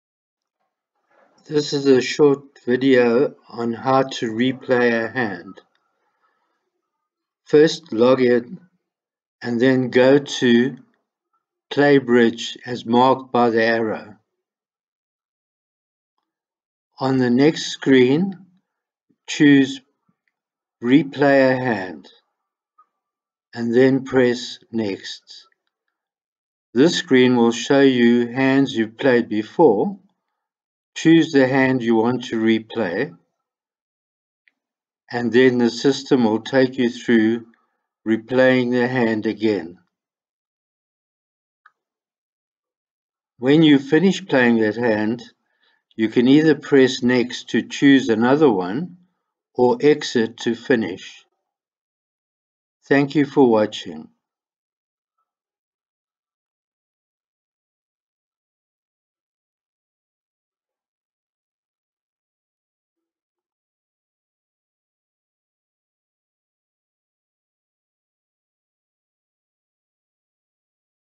Bridge - Replay hands - practicing and improving your declarer play with system
is a web site that allows you to practice your bridge declarer play. While you play bridge hands, it asks you to make choices. Then it explains what the best play would be and scores your answers. You can replay hands as many times as you like until you perfect your declarer play technique.